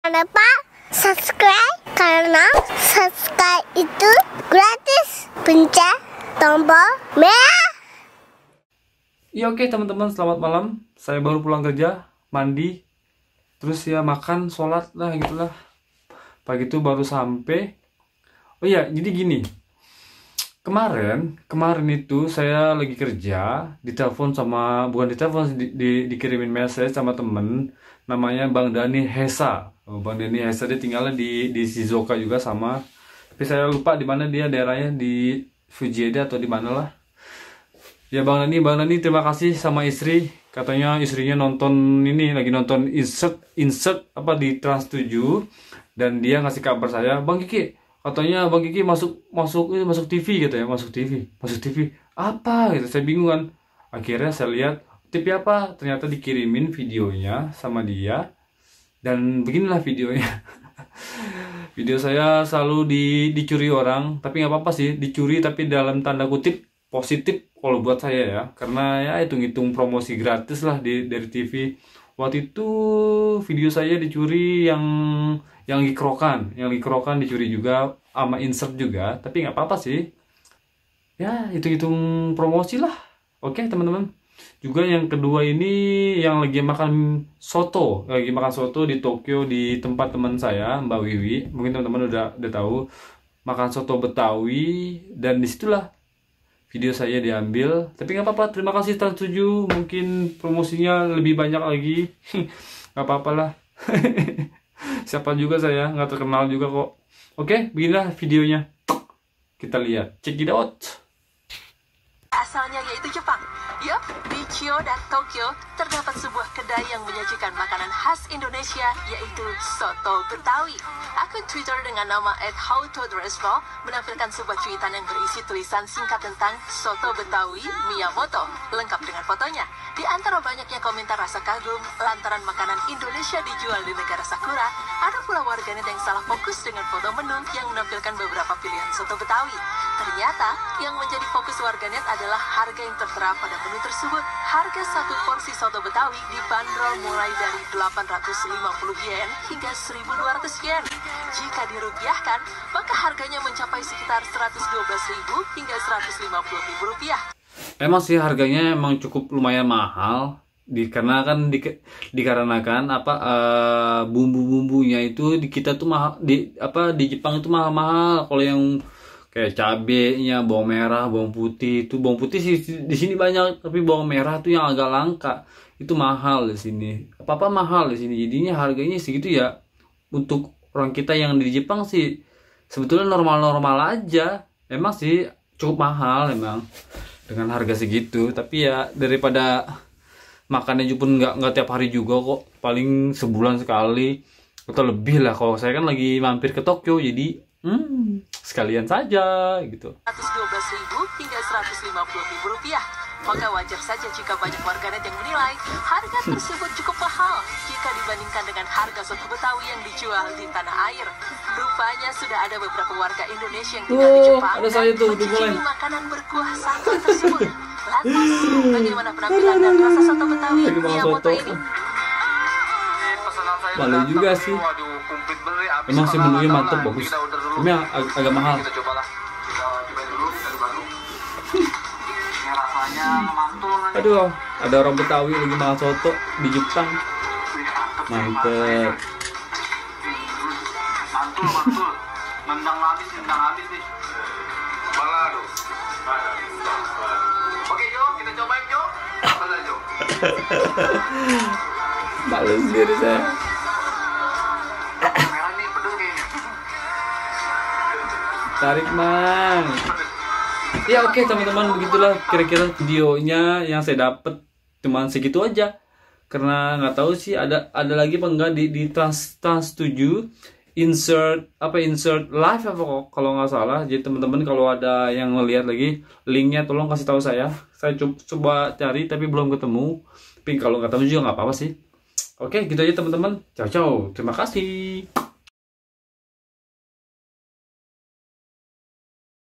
REACTION VIDEO MAKAN SOTO BETAWI MASUK TRANS TV
TERIMAKASIH BUAT BANG DANNY HEXA DAN ISTRI  YANG UDAH KASIH KABAR DAN REKAM VIDEONYA :)